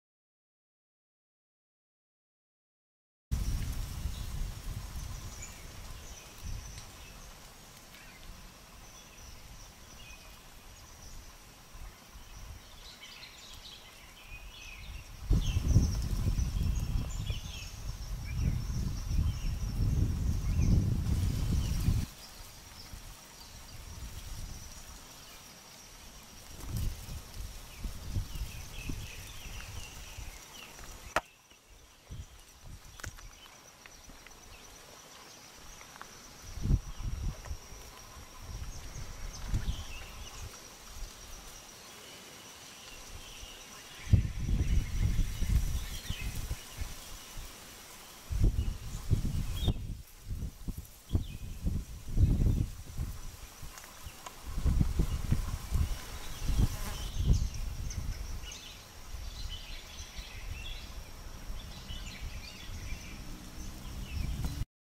Cynorkis purpurascens La Reunion
Cynorkis purpurascens La Reunion  ter hoogte van Les Trois Bassins D14. Gewoon langs de wegkant. Januari 2012